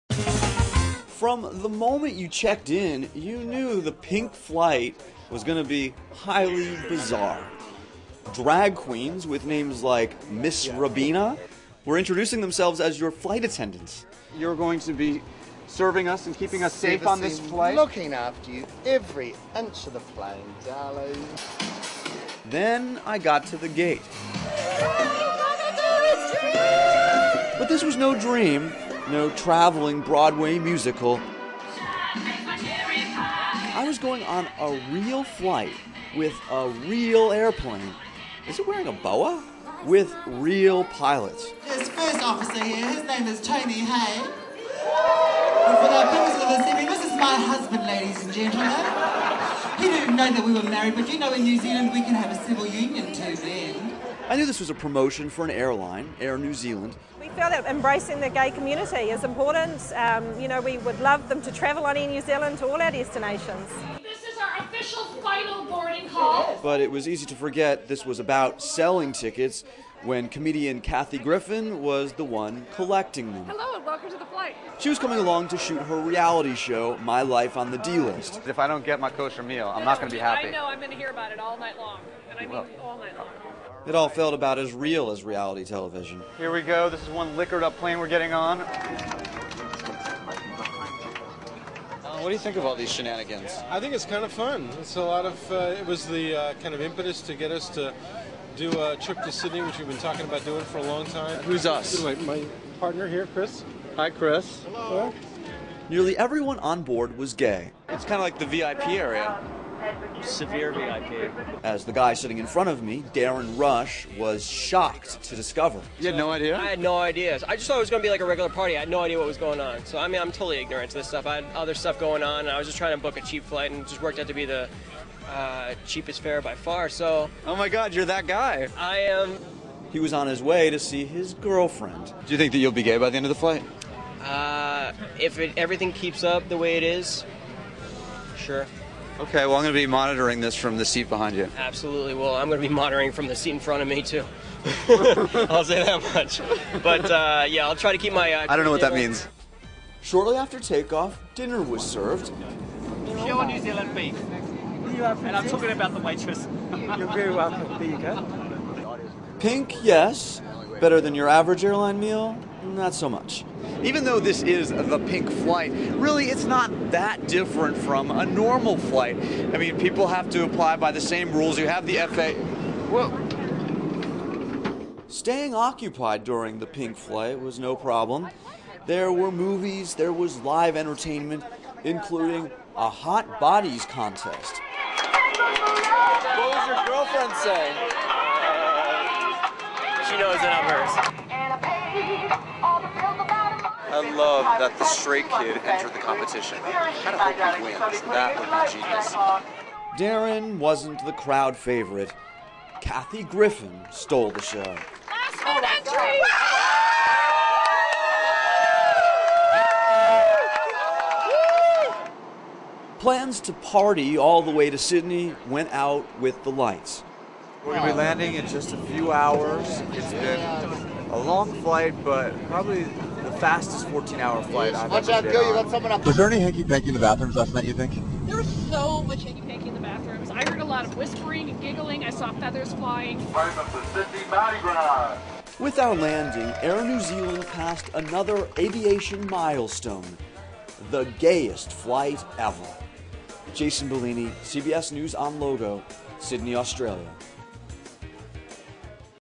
Air New Zealand Pink Flight
Jason Bellini and crew go to Australia on the Pink Flight with Kathy Griffin